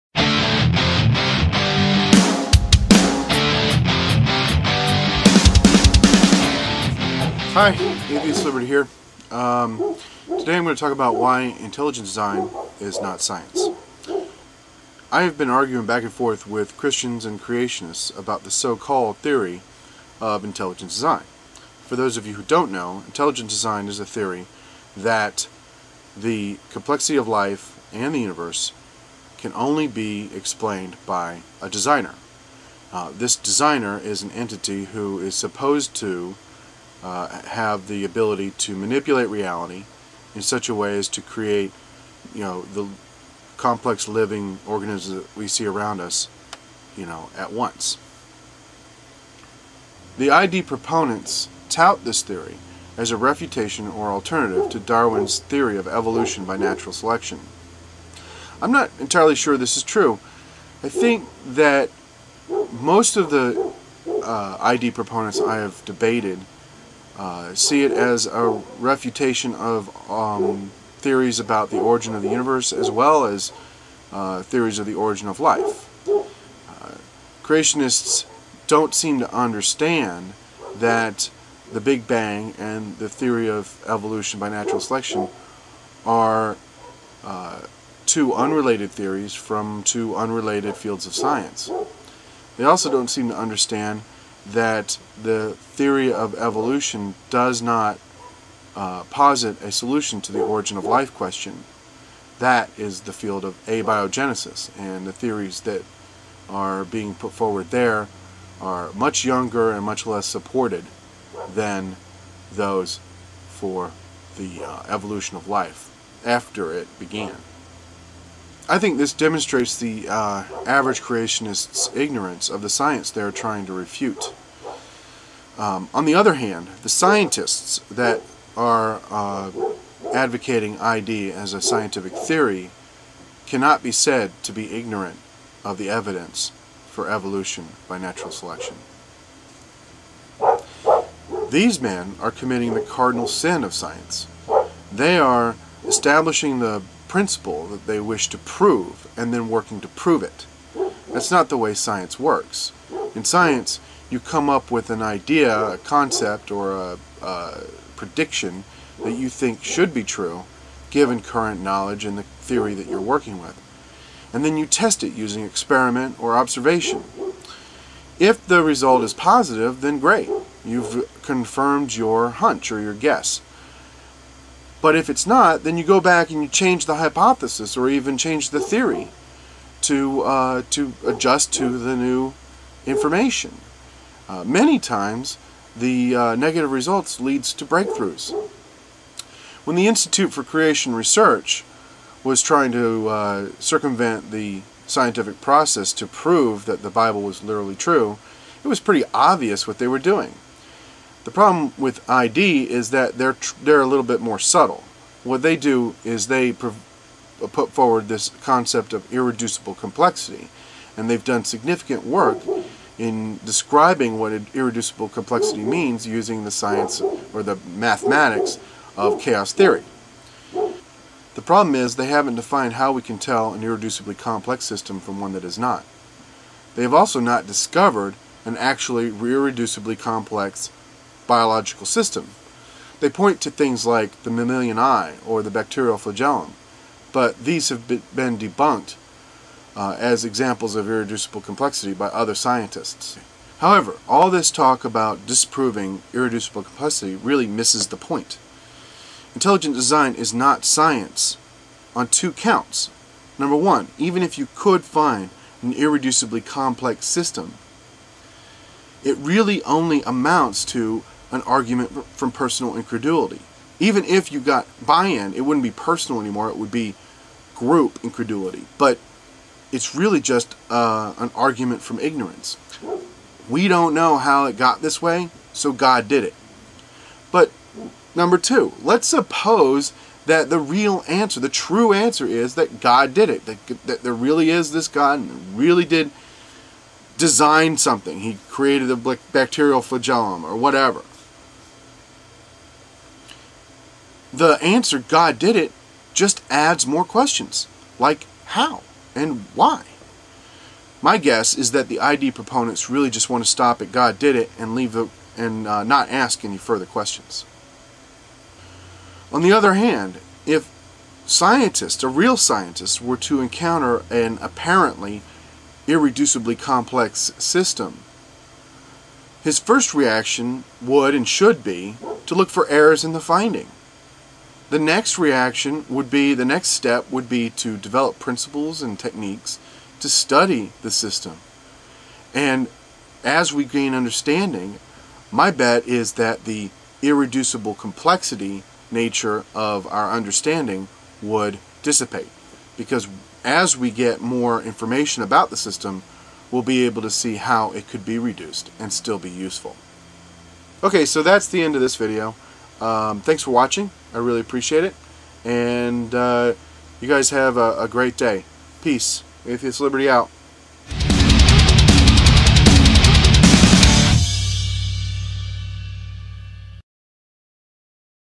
Intelligent Design is Not Science
Why Intelligent Design is Not Science
by Atheist Liberty

I have been arguing back and forth with several Christians and Creationists about the so called "Theory of Intelligent Design".  For those of you not familiar with it.  Intelligent Design is the theory that the complexity of life and the universe can only be explained by a designer.  This designer is supposed to be an entity that can manipulate reality in such a way as to create living beings in all their complexity.  I hope I have given a fair summary definition as I am not attempting to create a Strawman.

The ID proponents taut the theory as being a refutation or alternative to Darwin's Theory of Evolution by Natural Selection.  I do not think this is actually true.  It appears to me that most ID proponents think it replaces theories about the origin of life and theories about the origin of the Universe as well.  I find that many of the Creationists that I debate do not understand that Evolution and the Big Bang are two unrelated theories from two unrelated fields of science.  They also seem to miss the fact that Evolution does not, in any way, posit a solution to the origin of life question.  I think this demonstrates the average Creationists ignorance on the science they are attempting to refute.  On the other hand, the "scientists" that advocate ID cannot be said to be ignorant of the science that supports the Theory of Evolution by Natural Selection.

These men are actually committing the cardinal sin of science.  They are defining the principal that they wish to "prove" and working to prove it.  Science does not work that way.  In Science you come up with something you think should be true, given current knowledge, and then you test it with experiment or observation.  If it is shown to be correct, great, but if it is not, you adjust the hypothesis or the theory that it was drawn from and try again.  In some cases, a negative result leads to breakthroughs.

When the Institute for Creation Research was attempting to circumvent the scientific process to "prove" that the Bible is literally true, it was pretty obvious what they were doing.  In the case of ID the problem is a little more subtle.  They put forward the concept of "Irreducible Complexity" and have done significant work on describing what such a system would be like with mathematics from Chaos Theory.  The problem is they still haven't defined how we can tell an "Irreducibly Complex" system from one that is not.  They also have not yet discovered an example of an "Irreducibly Complex" system.  They point to things like the Mammalian Eye or the Bacterial Flagellum but these have been debunked as examples of "Irreducible Complexity" by scientists.

However, all this talk about "disproving" "Irreducible Complexity" misses the point.  ID fails to be science on two counts.  One, even if one could find an "Irreducibly Complex" system, by their definition, it really only amounts to an argument from personal incredulity.  aka. The Argument from Ignorance.  We don't know how this came to be, so "God Did It".  Two, the conclusion, "God Did It", does not actually answer the question.  It just adds new ones.  Let's suppose that the true answer is that "God Did It", the next question is "How?" and, possibly, "Why?".   My guess is that the ID proponent would just want to stop with "God Did It" and not ask any further questions.

On the other hand, if a scientist were to encounter an, apparently, "Irreducibly Complex" living system, their first reaction would be to look for errors in the finding.  Once satisfied that there are none the next step is to try and develop new principals and techniques for understanding the system.  In the process, the "Irreducible Complexity" of the system will probably dissipate since greater understanding allows us to discover the ways in which the system can be reduced and still be useful.